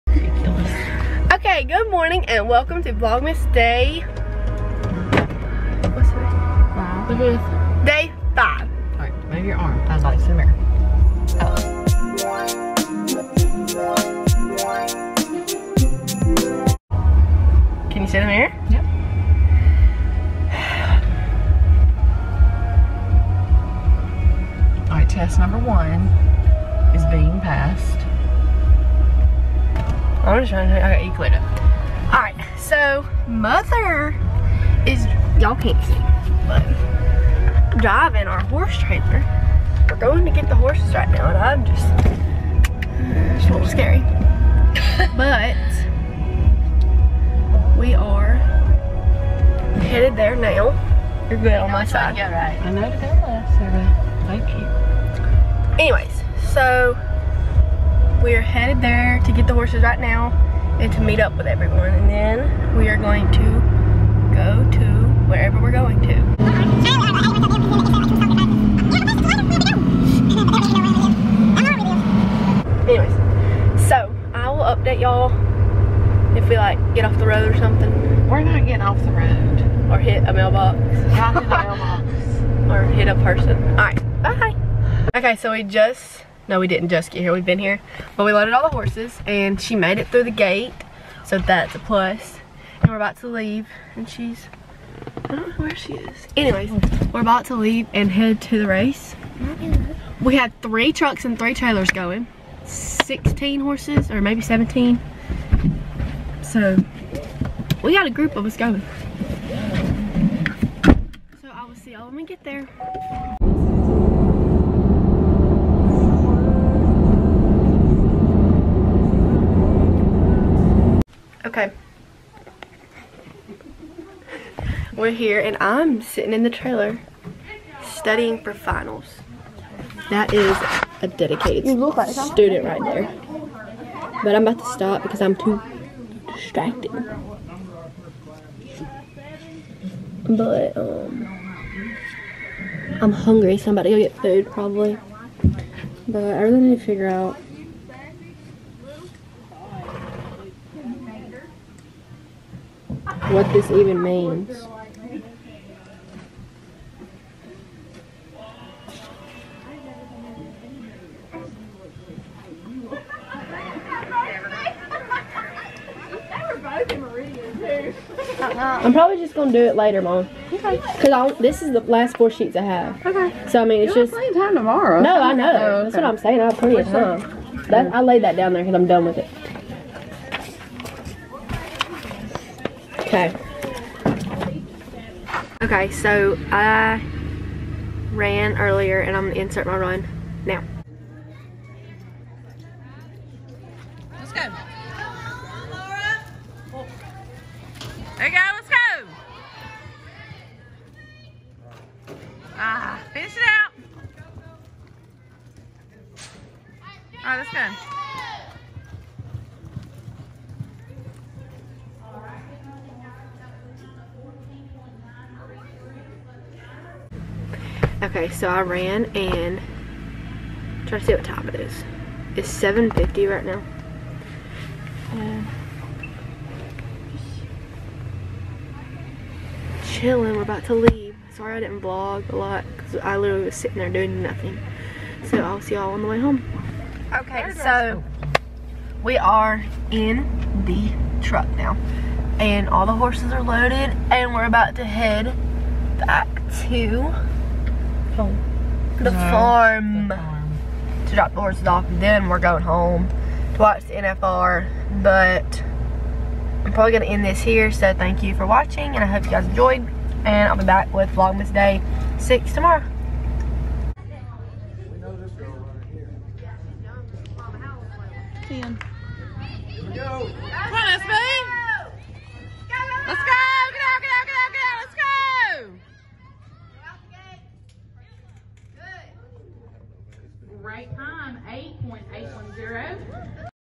Jack pot and my mom hauling for the first time!!!! Vlogmas day 5 | Laura Boothe
Thank y’all so so much for watching!!! Starting around mid this week I’ll be able to do much more interesting videos since I’ll be on Christmas break!